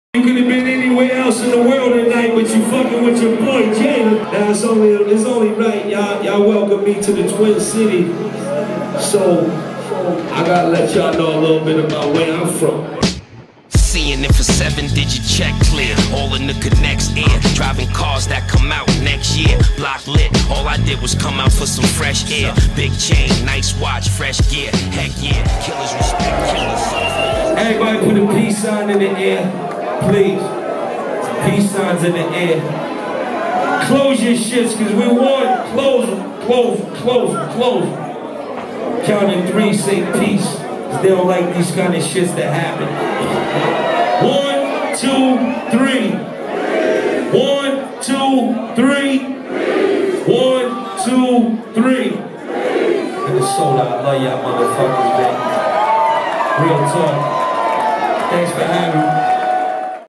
Jadakiss @ The Cabooze (Morningsidefilms.com)
Jadakiss performing at the Cabooze in Minneapolis.

Video by @Morningsidefilms [Instagram]
@Morningsidefilm [Twitter]
MorningsideFilms.com